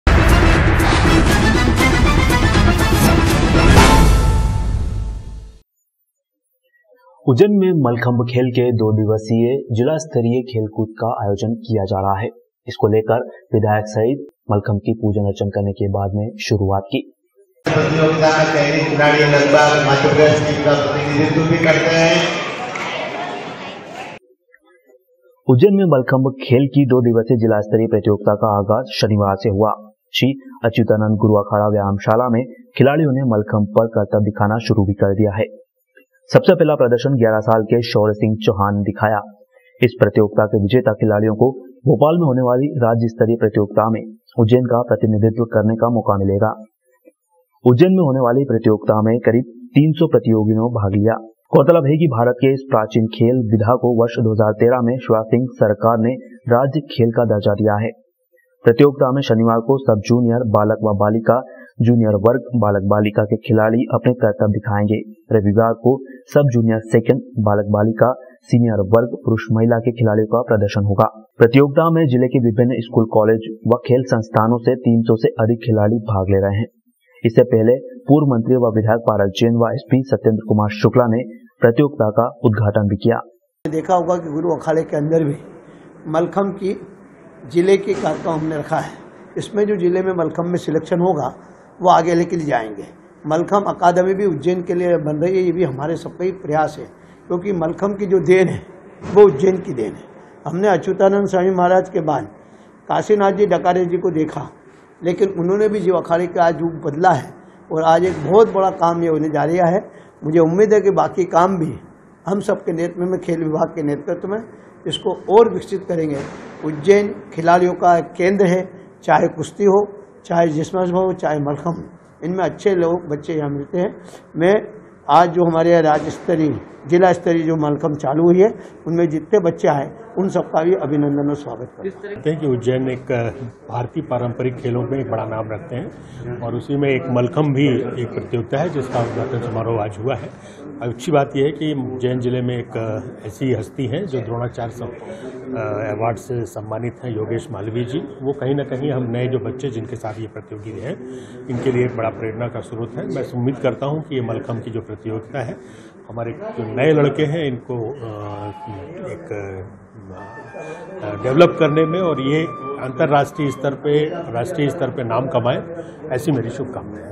उज्जैन में मलखंब खेल के दो दिवसीय जिला स्तरीय प्रतियोगिता का आयोजन किया जा रहा है
उज्जैन के एसपी और विधायक पारस जैन ने  मलखान का पूजन कर प्रतियोगिता शुभारंभ करवाई उज्जैन और खाचरोद के करीब 300 मलखंब खिलाड़ी इस प्रतियोगिता में भाग ले रहे हैं। प्रतियोगिता में विजेता भोपाल में होने वाली राज्य स्तरीय प्रतियोगिता में शामिल होंगे।

उज्जैन में मल्लखंभ खेल की दो दिवसीय जिला स्तरीय प्रतियोगिता का आगाज शनिवार से हुआ। श्री अच्युतानंद गुरु अखाड़ा व्यायामशाला में खिलाड़ियों ने मलखंभ पर करतब दिखाना शुरू कर दिया है। सबसे पहला प्रदर्शन 11 साल के शौर्य सिंह चौहान ने दिखाया। इस प्रतियोगिता के विजेता खिलाड़ियों को भोपाल में होने वाली राज्य स्तरीय प्रतियाेगिता में उज्जैन का प्रतिनिधित्व करने का मौका मिलेगा। उज्जैन में होने वाली प्रतियोगिता में करीब 300 प्रतियोगी ने भाग लिया है उज्जैन उज्जैन और खाचरोद के खिलाड़ियों ने प्रतियोगिता में जोर आजमाइश की गौरतलब है कि भारत के इस प्राचीन खेल विधा को वर्ष 2013 में शिवराज सिंह सरकार ने राज्य खेल का दर्जा दिया।  प्रतियोगिता में शनिवार को सब जूनियर (बालक व बालिका) और जूनियर वर्ग (बालक-बालिका) के खिलाड़ी अपने करतब दिखाएंगे। रविवार को सब जूनियर सेकंड (बालक-बालिका) और सीनियर वर्ग (पुरुष-महिला) के खिलाड़ियों के प्रदर्शन होंगे। प्रतियोगिता में जिले के विभिन्न स्कूल-कॉलेजों और खेल संस्थानों से 300 से अधिक खिलाड़ी भाग ले रहे हैं। इससे पहले पूर्व मंत्री व विधायक पारस जैन और एसपी सत्येंद्र कुमार शुक्ल ने प्रतियोगिता का उद्घाटन किया।

बाइट--सत्येंद्र कुमार शुक्ल एसपी

बाइट--पारस जैन, विधायक